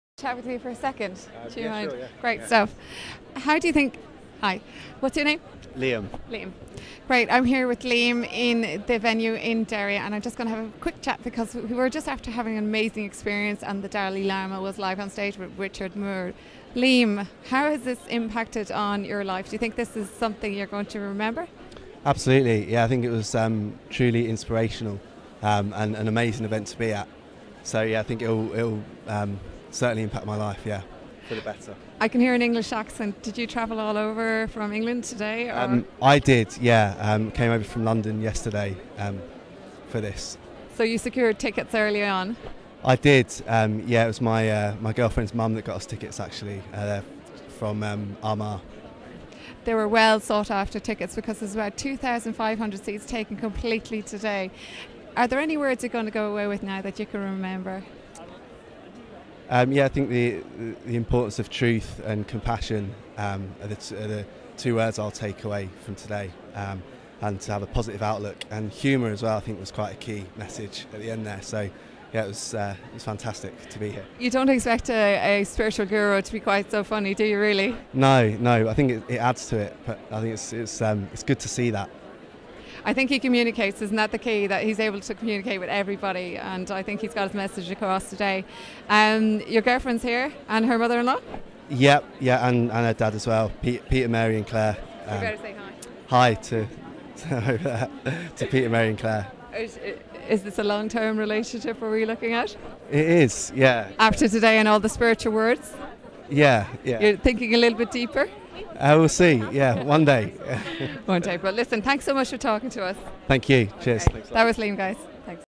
Guests talk to Marian ShanleyWest after the Dalai Lama addresses public in Derry Londonderry
Marian ShanleyWest talks to Liam after the Dalai Lama addresses over 2,500 people in Derry Londonderry after the official opening of The Peace Bridge spanning the River Foyle and uniting the Catholic and Protestant sides of the City.